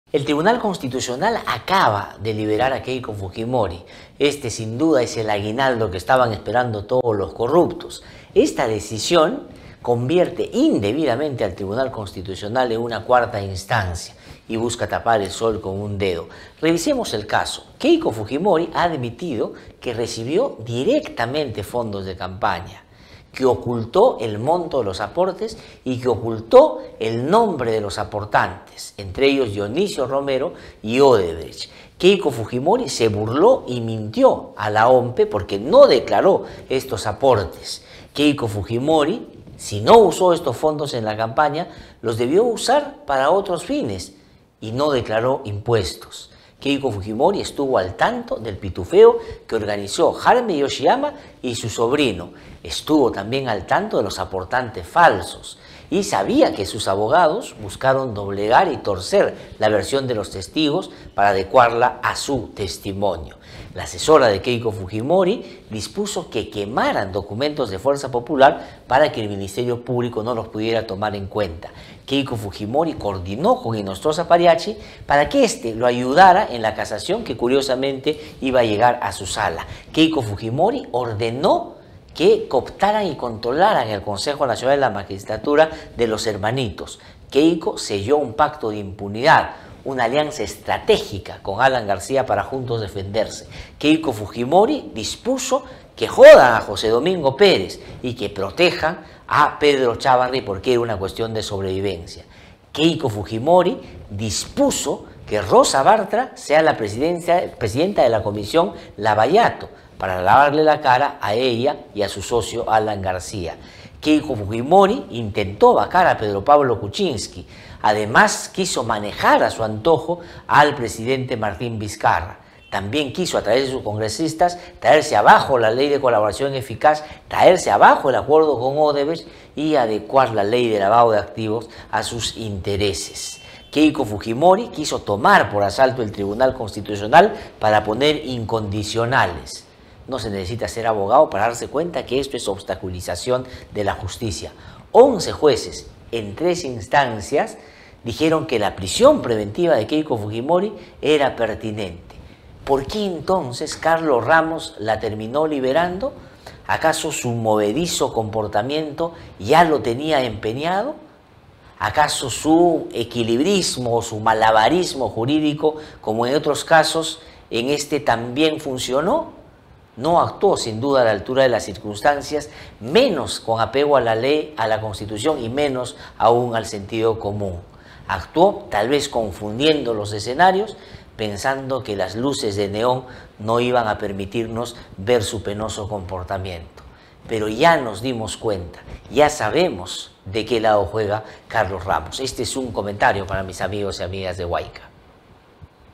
"Liberación de Keiko es el aguinaldo de los corruptos"
A pesar de las pruebas de obstaculización de la justicia, el Tribunal Constitucional decidió anular la prisión preventiva contra Keiko Fujimori.

El periodista Glatzer Tuesta de Ideeleradio te explica lo sucedido en 3Minutos